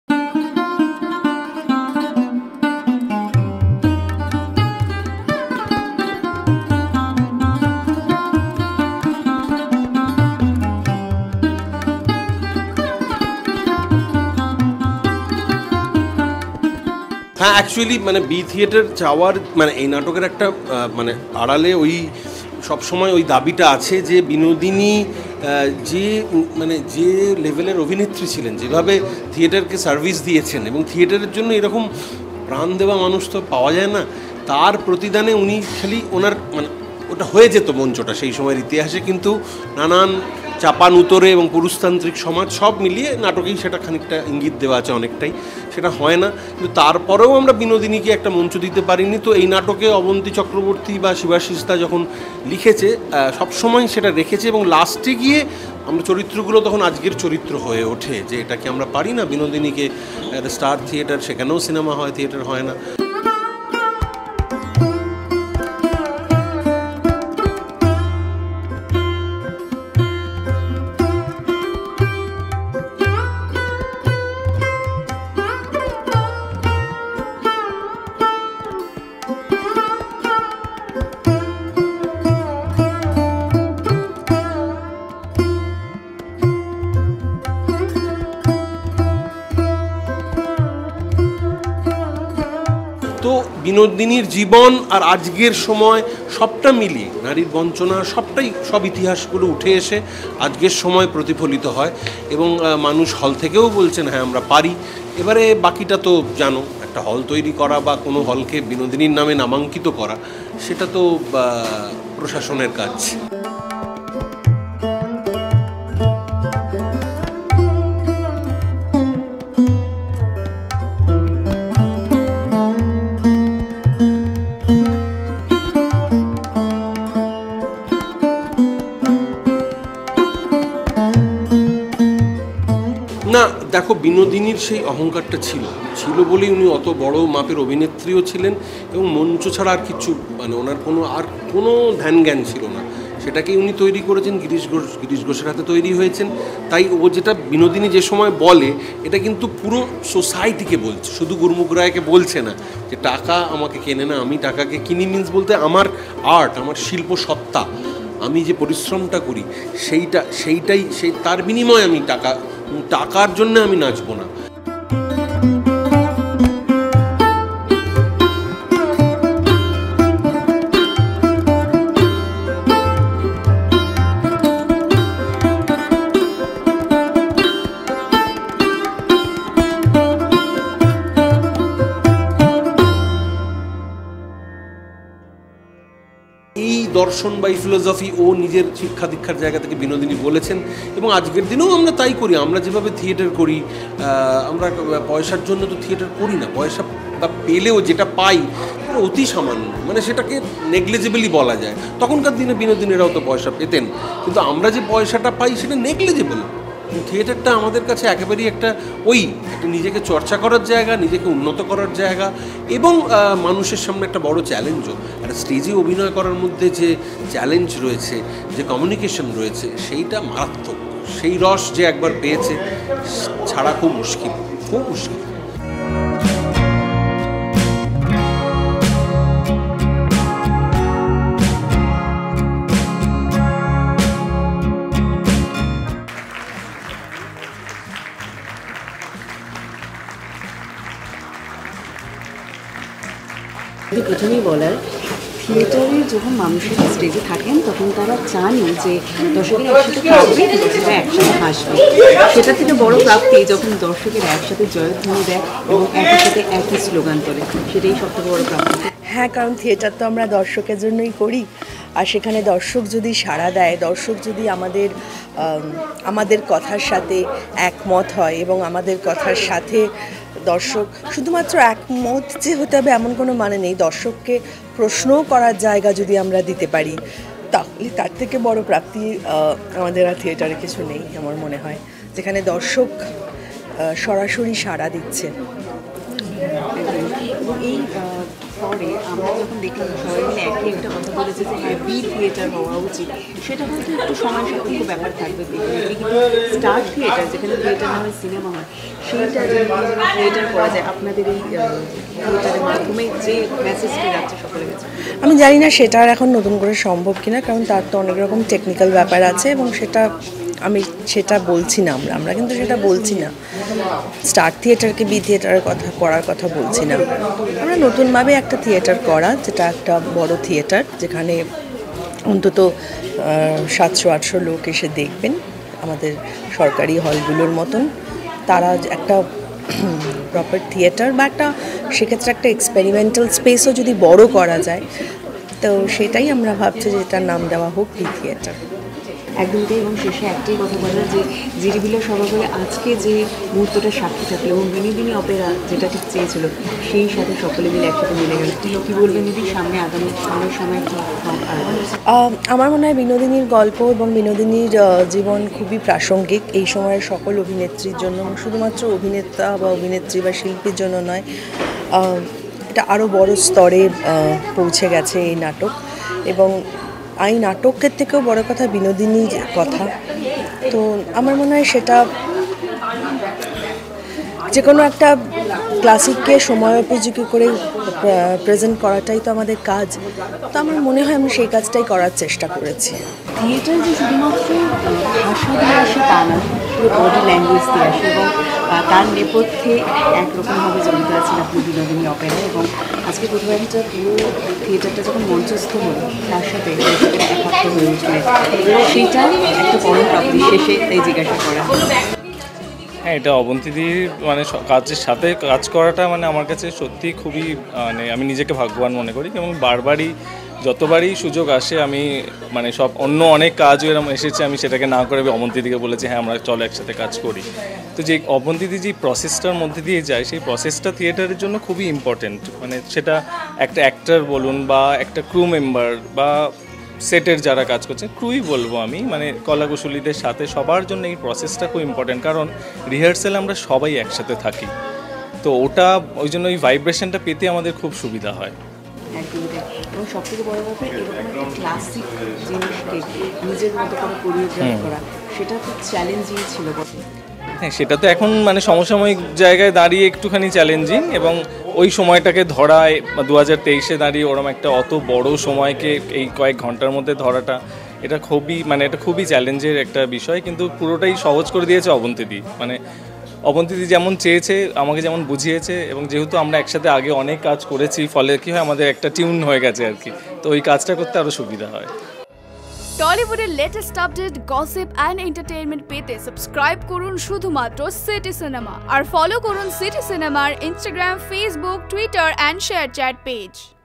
মঞ্চে নটীর চরিত্রে সুদীপ্তা | Sudipta Chakraborty | Binodini Opera | Sujan | Padmanava Dasgupta
Sudipta Chakraborty returns to the stage in the role of Noti Binodini. Noti Binodini, who is still relevant 81 years after her death. The actress mesmerized the audience with her powerful on stage acting in the role of Binodini Dasi in 'Binodini Opera'

মঞ্চে নটীর চরিত্রে সুদীপ্তা | Sudipta Chakraborty | Binodini Opera | Sujan Mukherjee | Padmanava Dasgupta